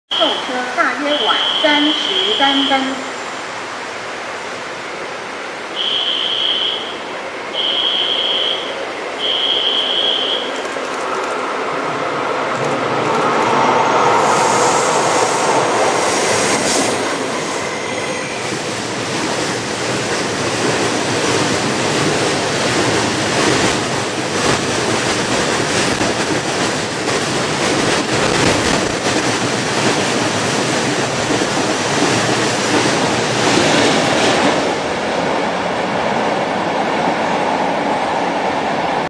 台鐵新型電力機車頭E500負重測試7527B次貨物列車汐科站通過-2024/02/27
去程不要中線.回程就可以XD  汐科好多人在等~